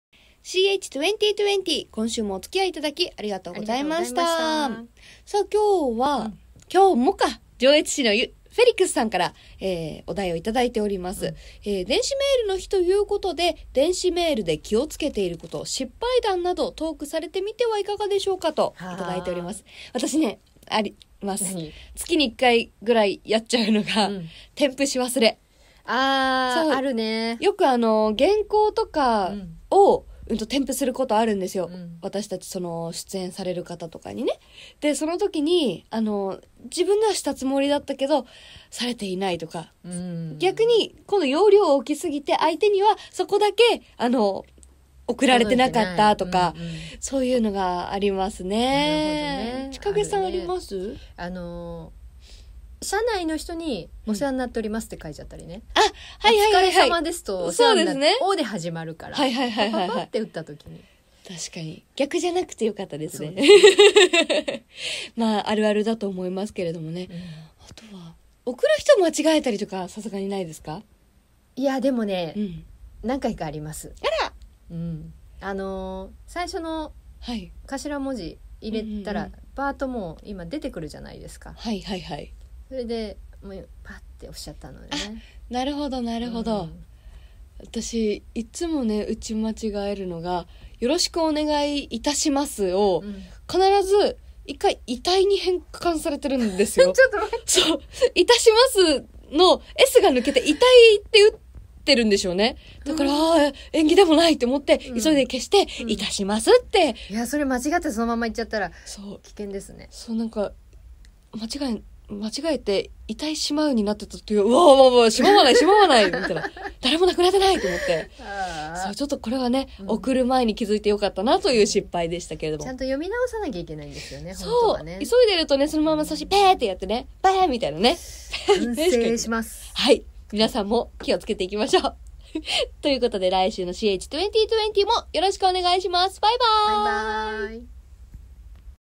メールでの失敗！
きょうは「電子メールの日」ということで、メールでの失敗談について話しています！